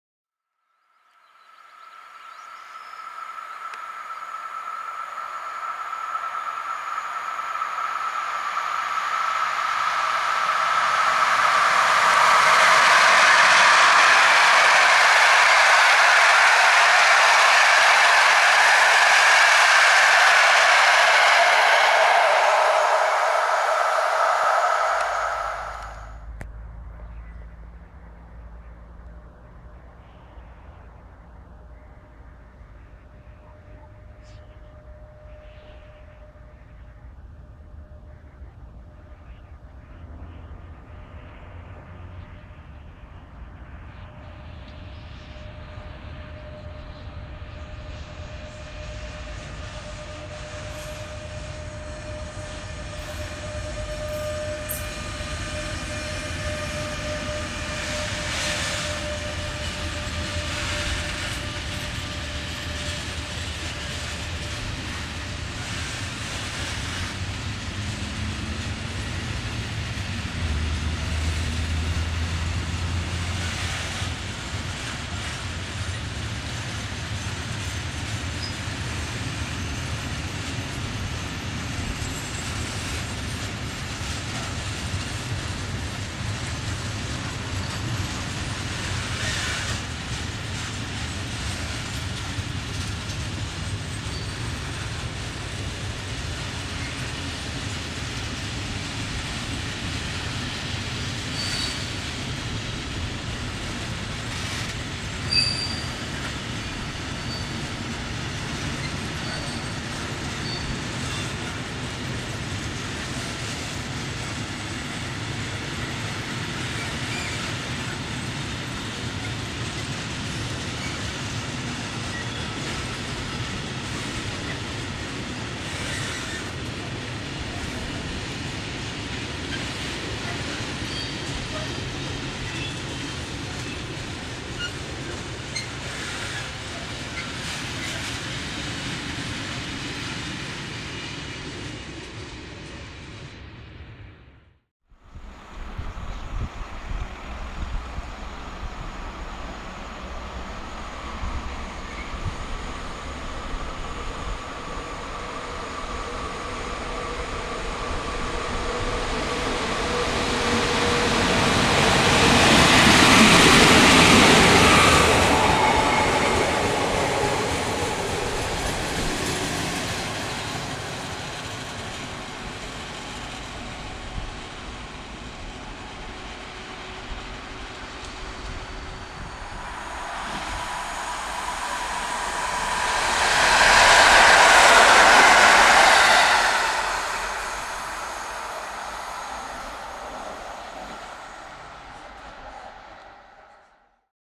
EP07-330, E186 273-9, EU07-1510, 14WE-004 - Mały Mix .
Film przedstawia przejazd czterech składów każdy innego przewoźnika na " E-65" :
0:00-0:24 EP07-330 na czele TLK 51106/51107 "WOJCIECHOWSKI" Gdynia Główna - Warszawa Zachodnia , pozdrowienia dla towarzysza Tomka , Stupsk 19/06/2016r.
0:24-2:25 E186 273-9 ze składem węglarek odjeżdża z Ciechanowa w kierunku Mławy 18/06/2016r.
2:25-2:59 EU07-1510 ze składem dwóch wagonów PKP PLK przejeżdża przez Mławę w kierunku Ciechanowa 24/06/2016r.
3:00-3:14 14WE-004 jako przejazd służbowy w kierunku Działdowa (niestety palec się wkradł ;/) Mława 24/06/2016r.